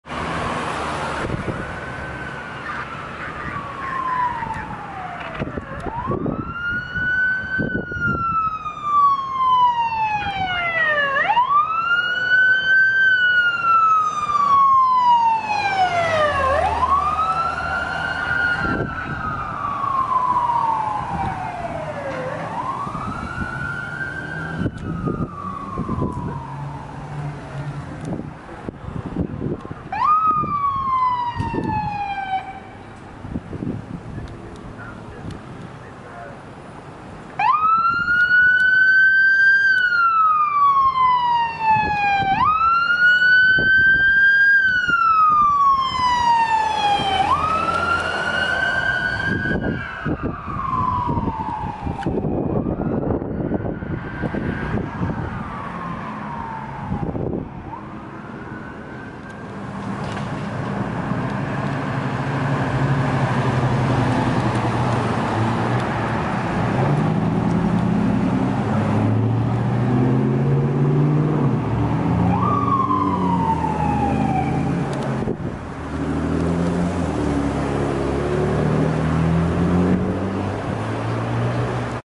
LAFD Rescue 64 & Rescue 264 Transporting
Los Angeles Fire Dept. Rescue 264 transports an unconscious person and Rescue 64 transports a person with chest pains to Martin Luther King Hospital after each handle seprate medical calls on 103rd St. x Compton Ave.

Location: Watts, Los Angeles, CA
Date: Wednesday, March 29th, 2023